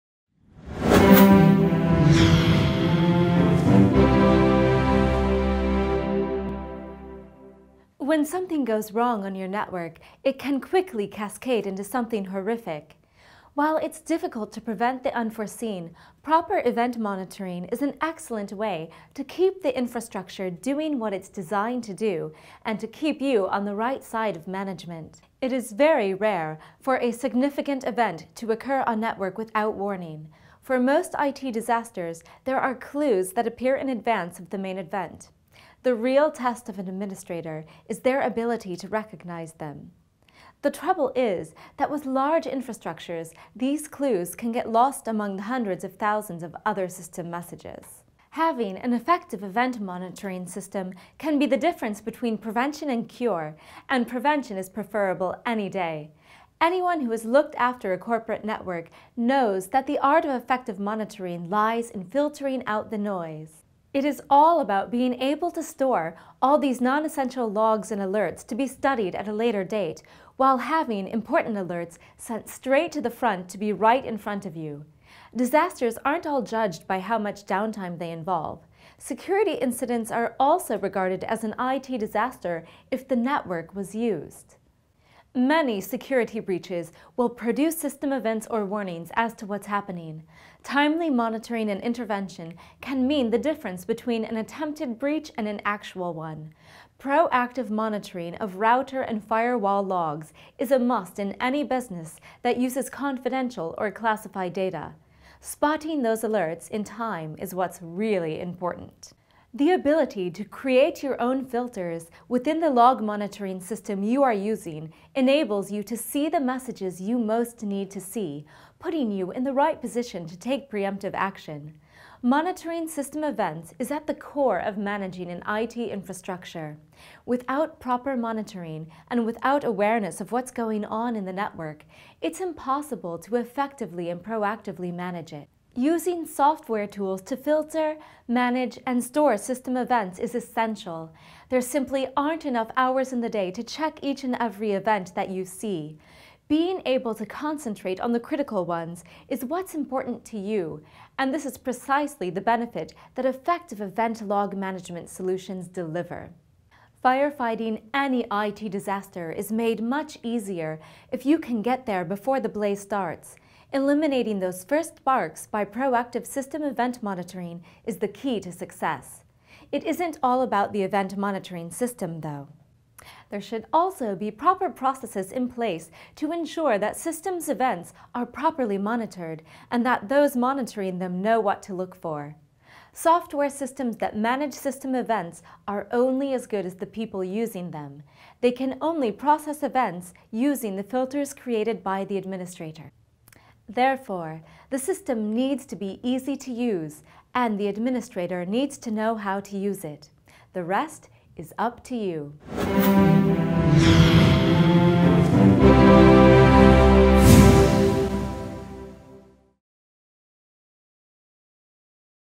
How event monitoring can prevent IT disasters
Knowing what's going on in your network is the best way to keep it all running smoothly. Being able to spot problems before they happen will make you a legend in the IT room and in the board room!
Using comprehensive system monitoring tools to filter, sort and display the millions of system events is an excellent way to stay on top of a busy network. These tools allow you to quickly assess the state of your network and spot problems or security breaches before or as they happen. By seeing and acting on warning signs, you can prevent them from becoming critical issues.